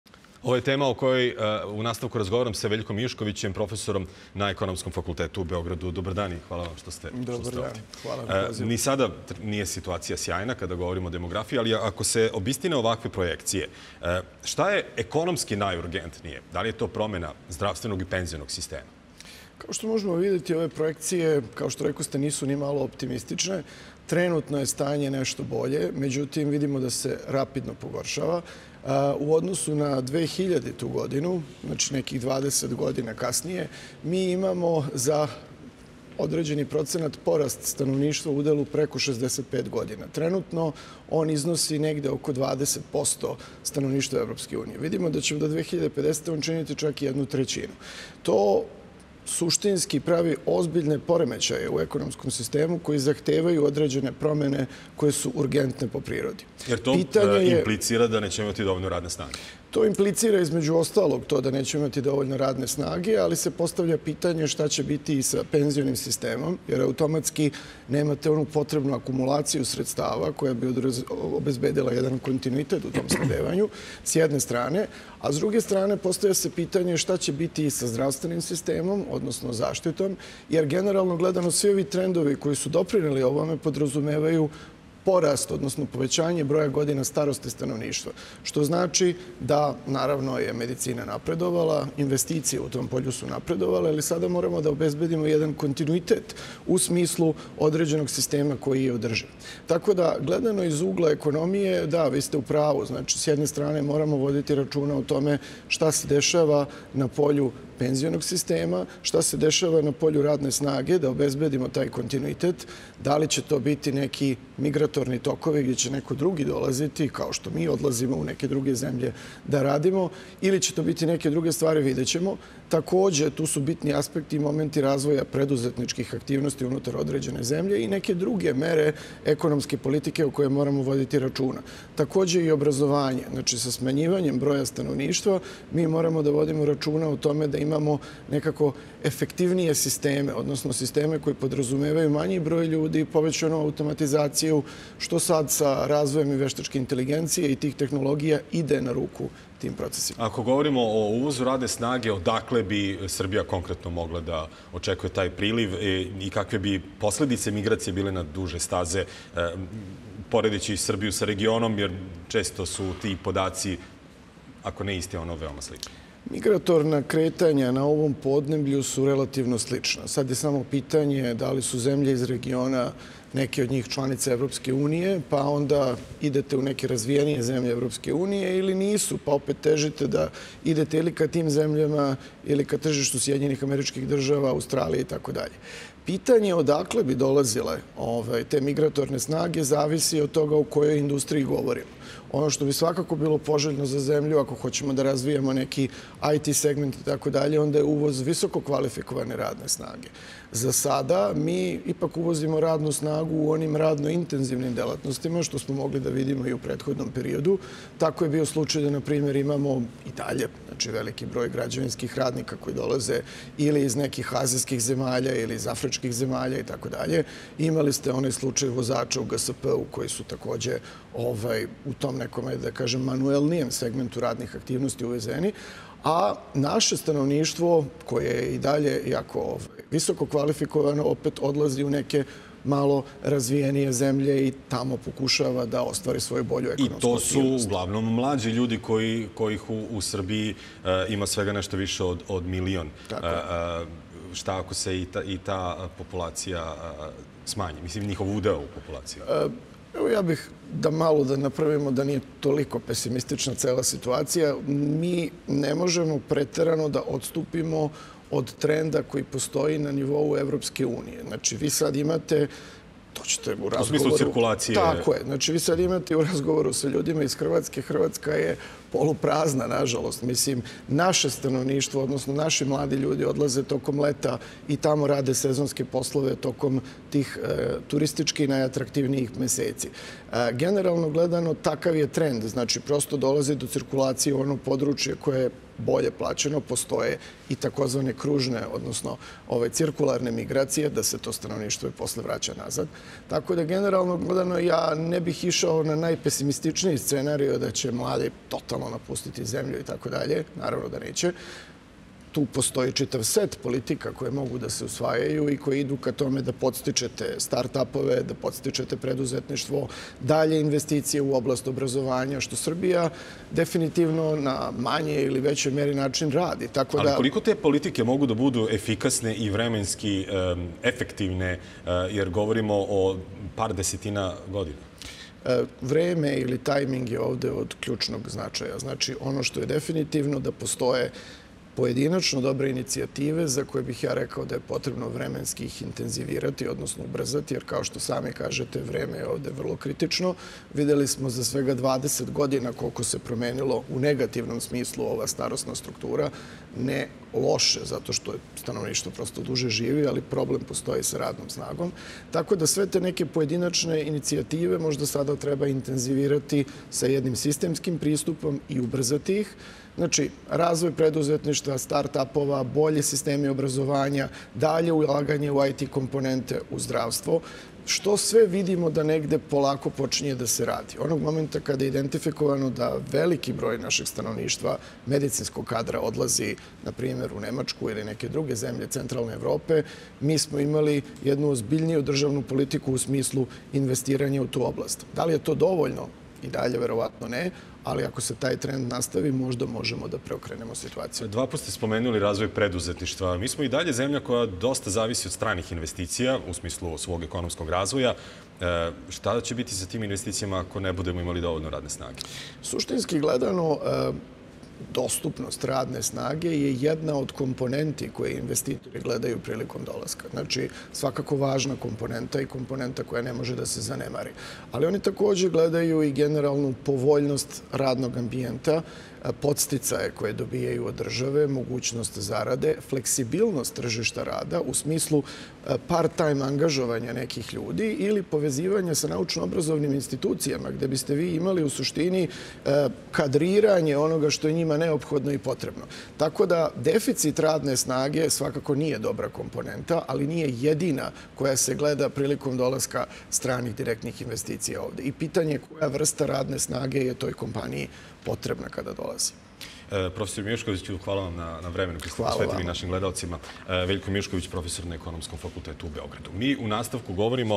Mijušković: Demografska situacija zahteva promene koje su po prirodi urgentne
Profesor Ekonomskog fakulteta u Beogradu, Veljko Mijušković izjavio je za Euronews da se demografska situacija drastično pogoršava i da to suštinski pravi ozbiljne poremećaje u ekonomskom sistemu koji zahtevaju određene promene koje su urgentne po prirodi. Dodaje da gledajući sa ugla ekonomije s jedne strane moramo voditi računa o tome šta se dešava na polju penzionog sistema, s druge strane na polju radne snage, da bi se obezbedio kontinuitet.